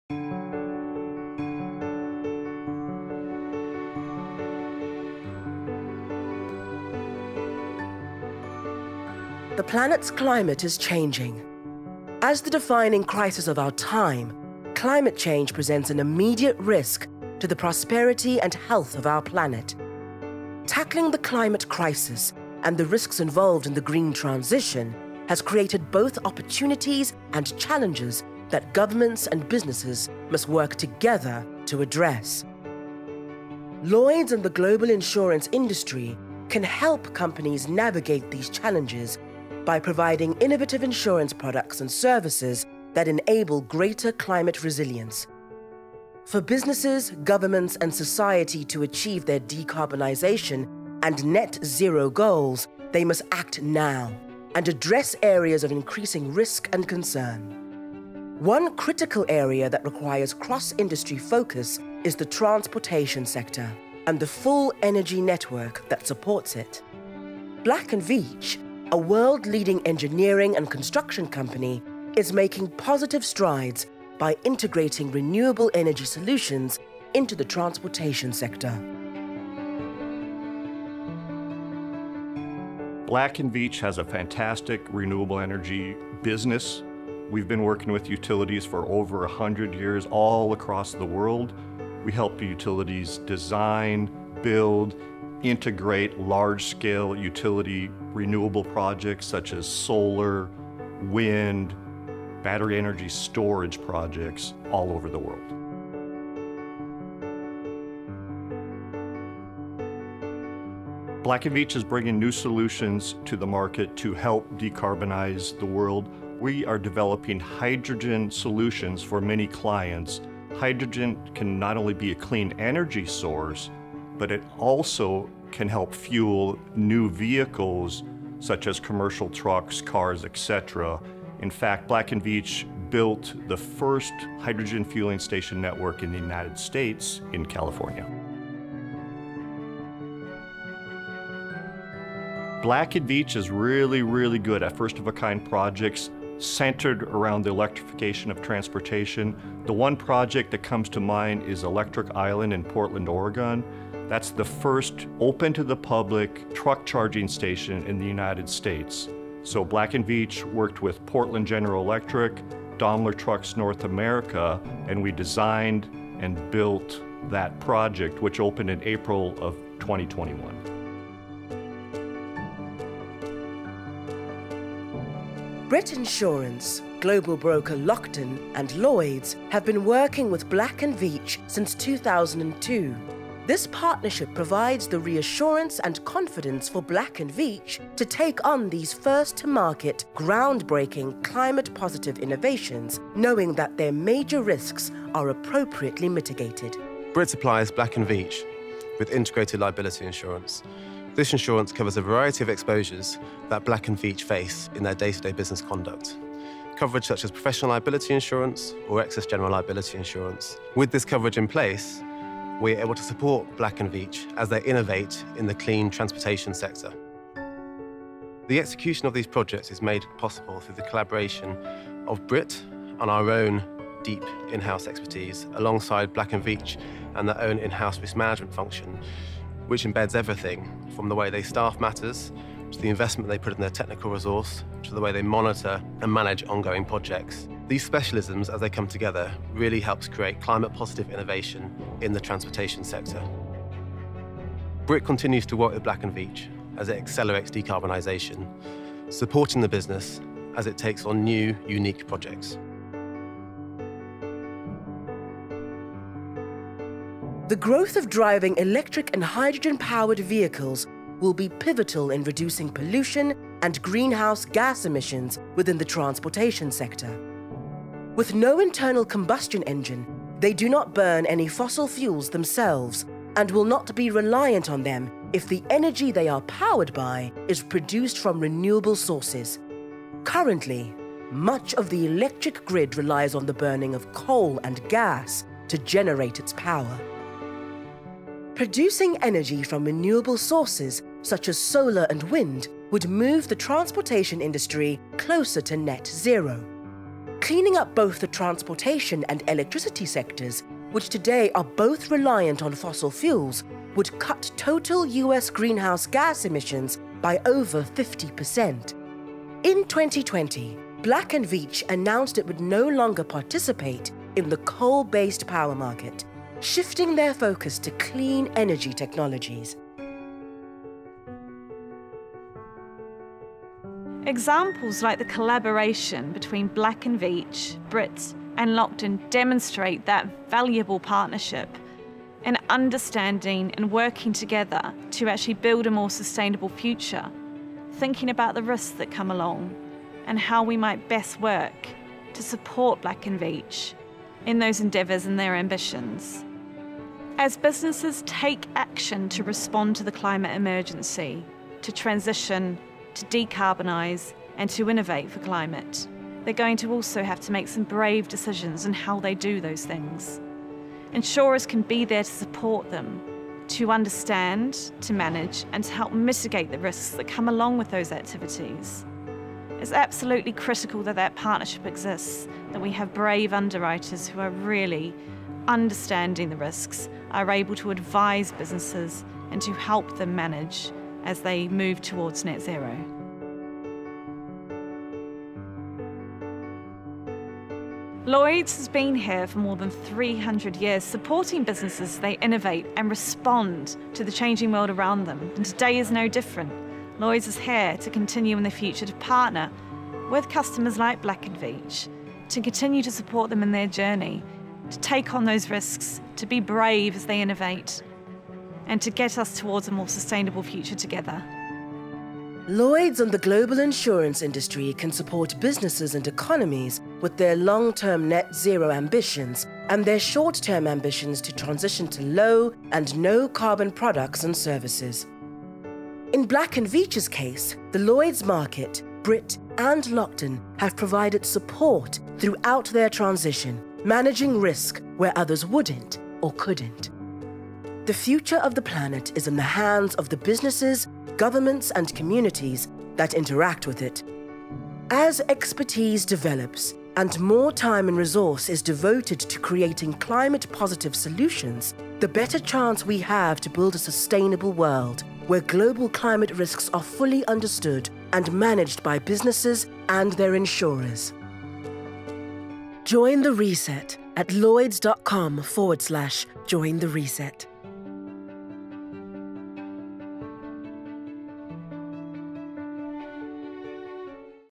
Building sustainable solutions for industry: Lloyd’s talks to Black & Veatch
To avoid climate catastrophe, we must act now.

Brave innovators, experts and leaders across the energy transportation and industry sectors are making difficult decisions and stepping forward in the face of adversity to accelerate the transition to a low-carbon economy.

Black & Veatch are an engineering and construction company building green energy solutions for industry through their large-scale renewable utility projects. In this video case study, we spoke with B&V’s team, alongside their supporters in the Lloyd’s market Brit insurance and global broker Lockton, to shine a light on their present and future sustainability projects.